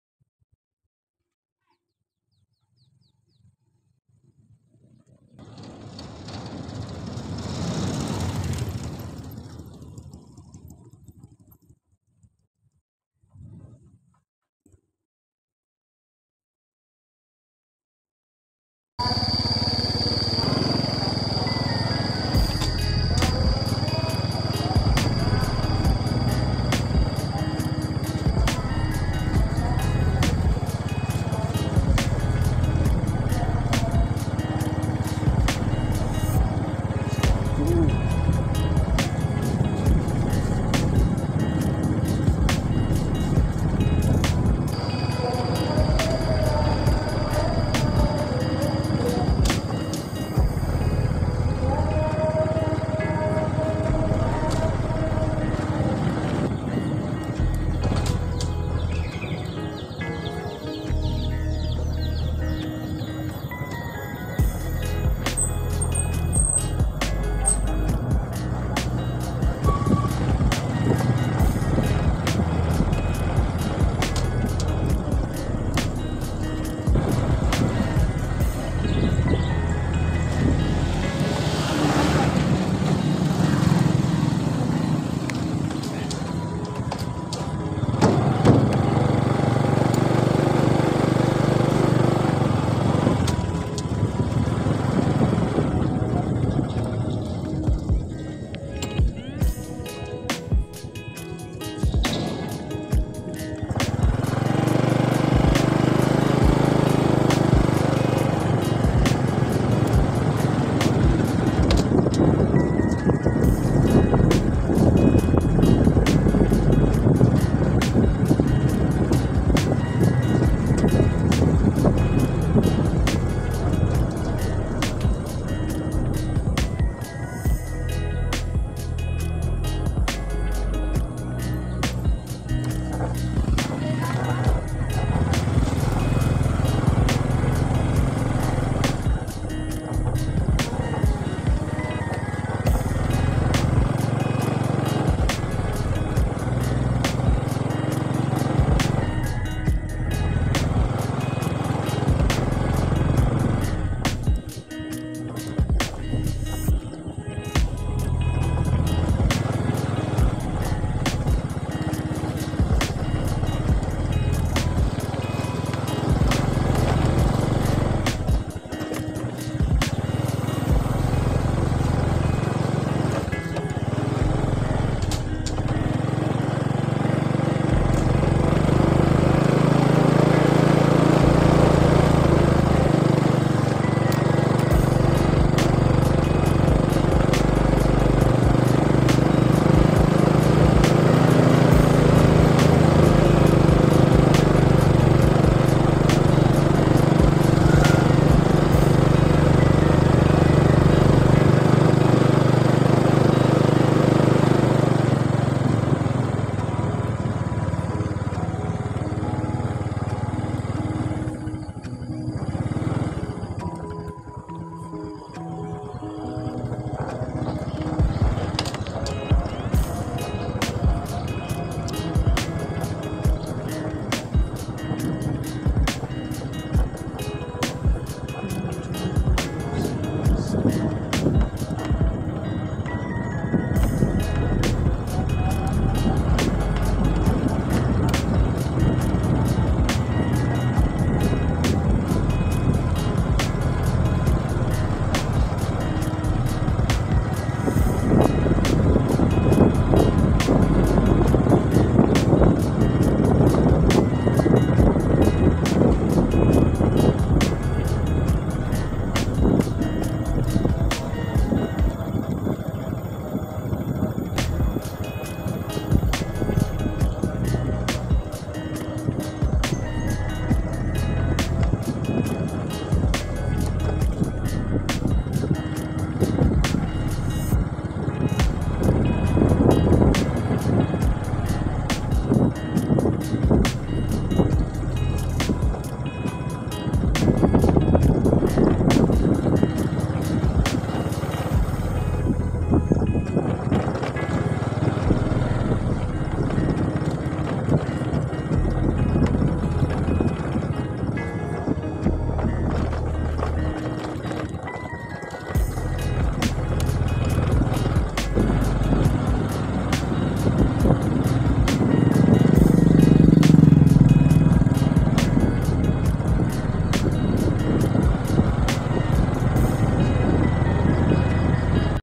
Jembatan Merah Citarum Indobarat yang Penuh Mistis
Jembatan merah citarum dibelakang pt indobarat pwk konon katanya menyeramkan suasana yang sangat sepi